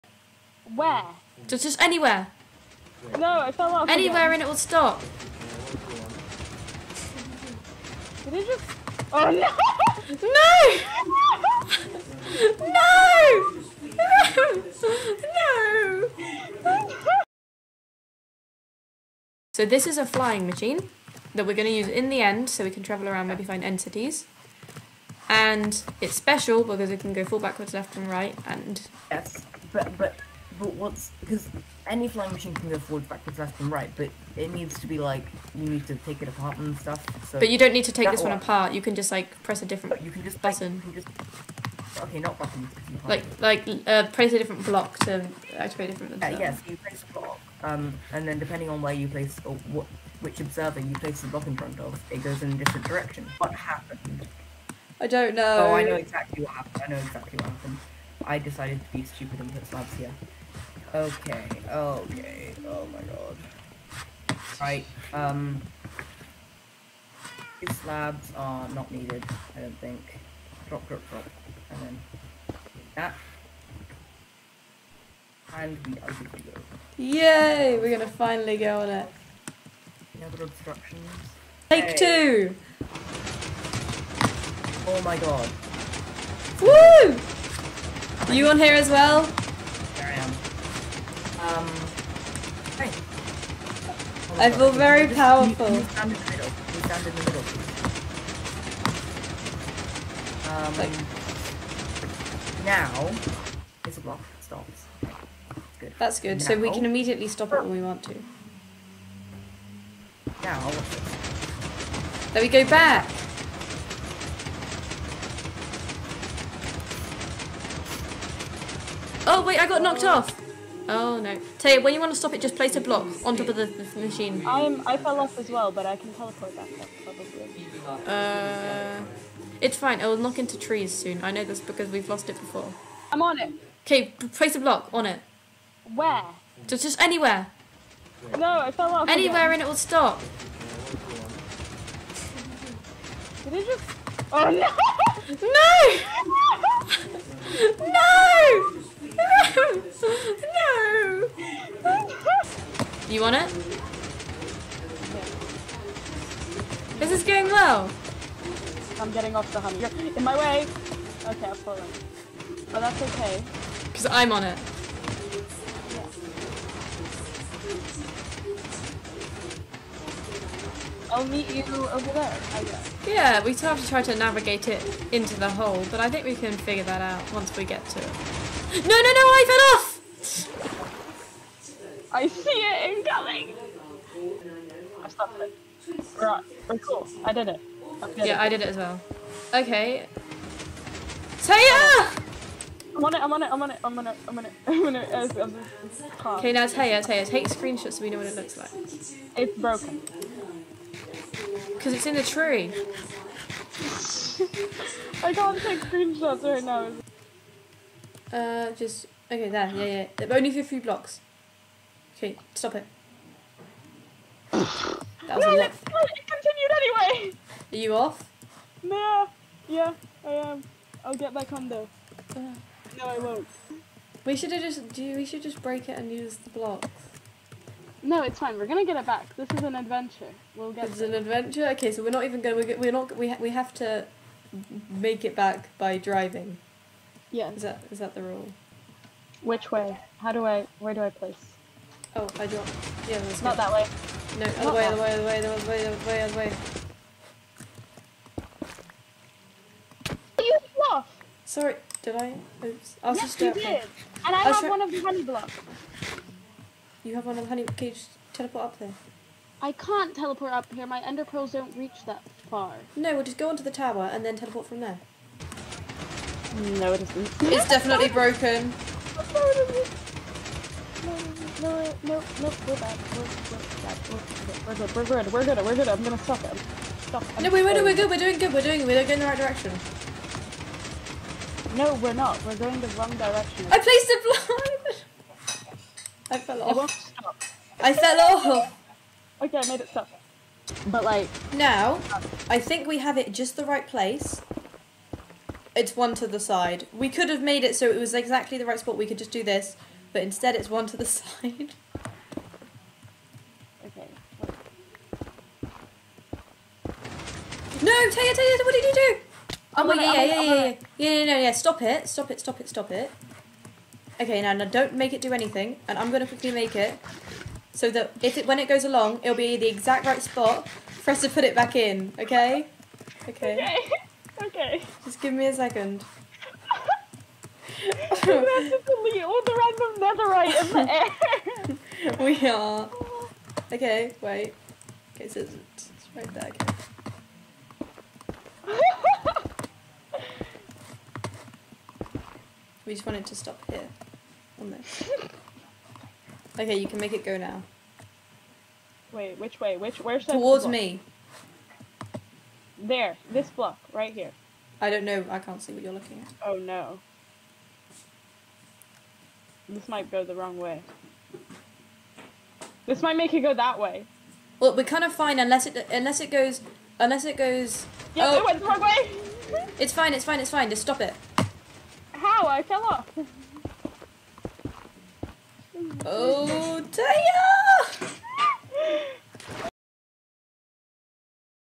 The Contraption
I built contraption with my friends (which was just a flying machine) and proceeded to fly it around our server and lose it a bunch of times. It was really fun recording this video so I hope you enjoy watching it.